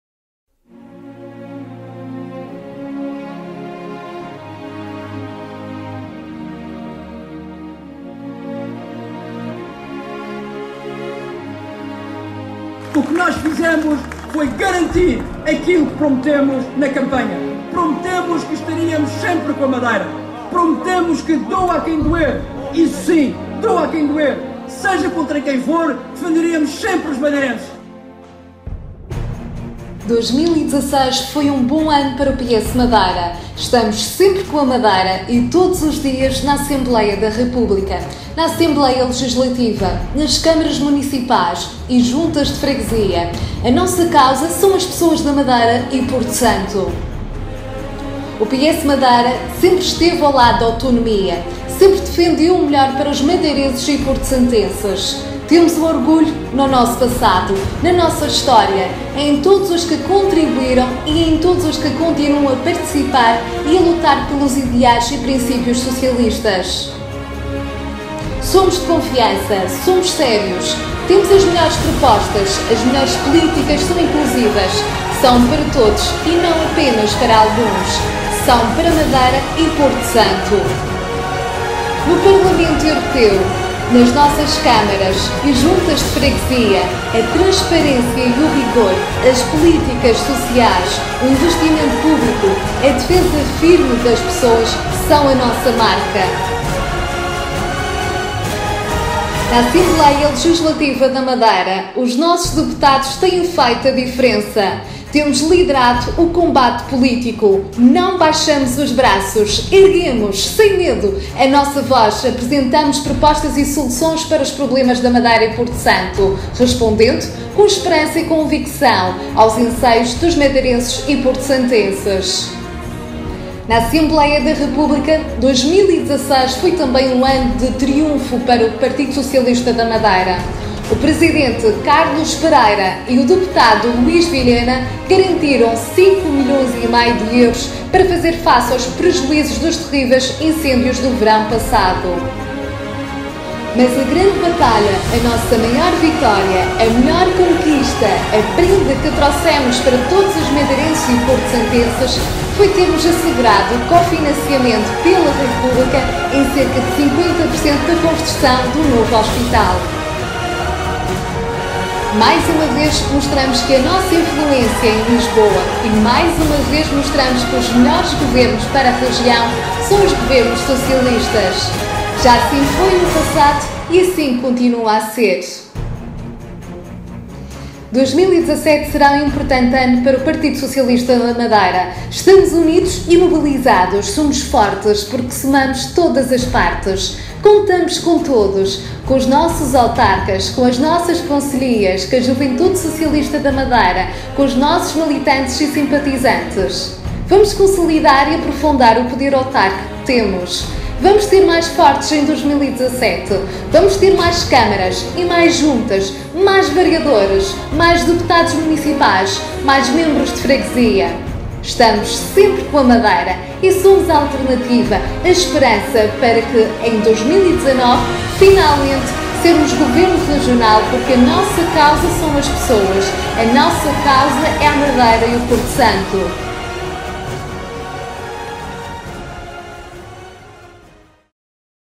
2016 EM REVISTA | 8 DEZ 16
Nós prestamos "contas" aos madeirenses e porto-santenses, apresentando assim o ano de 2016 em revista. Prometemos, estamos a cumprir.